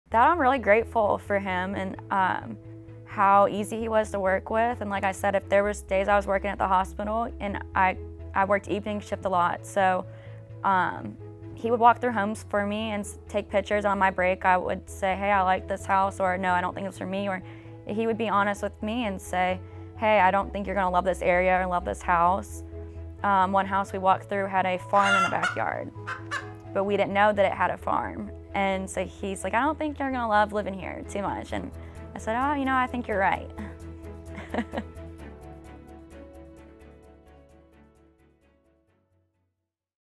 Why recommend Mike?
Lauren tells about why she appreciates the work Mike did for her on the purchase of her new home...